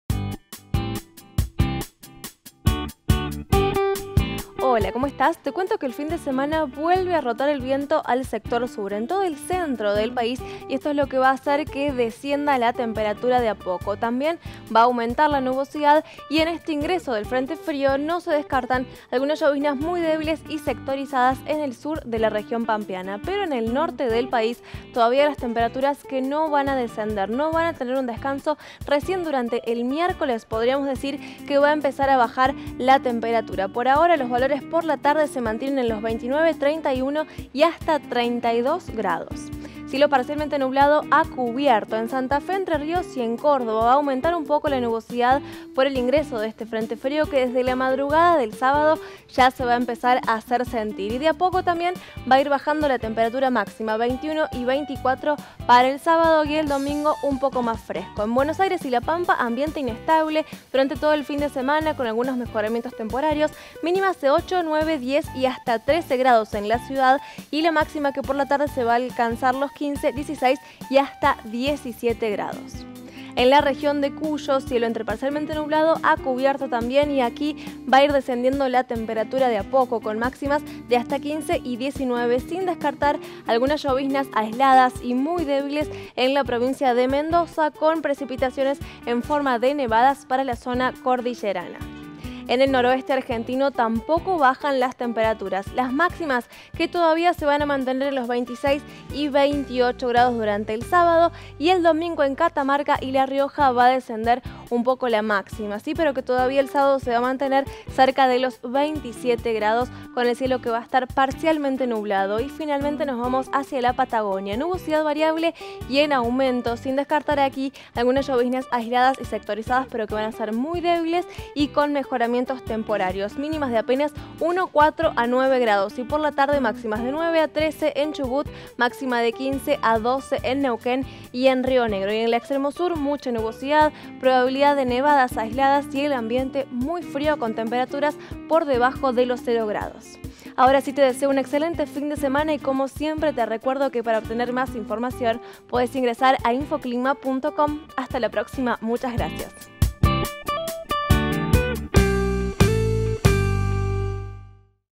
Pronóstico para el 29 y 30 de julio de 2017. Argentina - Infoclima TV
Presenta: Florencia Medina